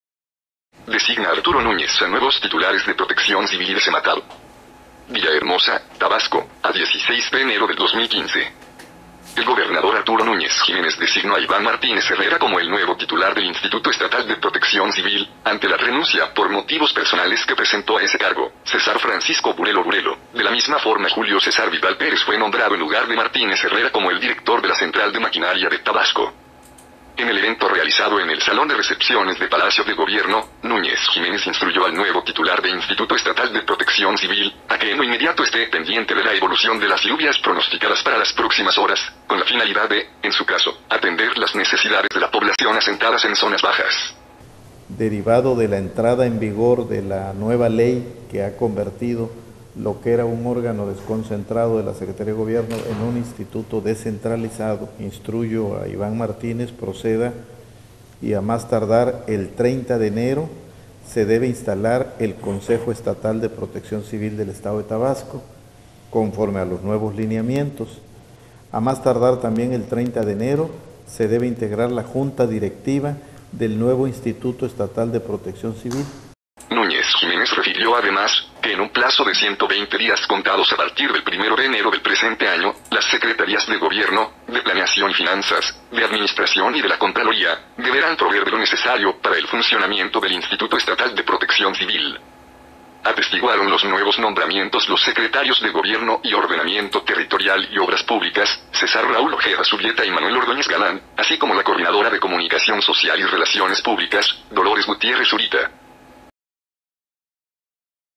Designa Arturo Núñez a nuevos titulares de Protección Civil y de Cematab
Villahermosa, Tabasco, a 16 de enero del 2015.

El gobernador Arturo Núñez Jiménez designó a Iván Martínez Herrera como el nuevo titular del Instituto Estatal de Protección Civil, ante la renuncia por motivos personales que presentó a ese cargo, Cesar Francisco Burelo Burelo; de la misma forma Julio Cesar Vidal Pérez fue nombrado en lugar de Martínez Herrera como el director de la Central de Maquinaria de Tabasco.

En el evento realizado en el Salón de recepciones de Palacio de Gobierno, Núñez Jiménez instruyó al nuevo titular de Instituto Estatal de Protección Civil, a que en lo inmediato esté pendiente de la evolución de las lluvias pronosticadas para las próximas horas, con la finalidad de, en su caso, atender las necesidades de la población asentadas en zonas bajas:…

Núñez Jiménez refirió además, que en un plazo de 120 días contados a partir del primero de enero del presente año, las Secretarías de Gobierno, de Planeación y Finanzas, de Administración y de la Contraloría, deberán proveer de lo necesario para el funcionamiento del Instituto Estatal de Protección Civil.

Atestiguaron los nuevos nombramientos los secretarios de Gobierno y Ordenamiento Territorial y Obras Públicas, Cesar Raúl Ojeda Zubieta y Manuel Ordoñez Galán, así como la coordinadora de Comunicación Social y Relaciones Públicas, Dolores Gutiérrez Zurita.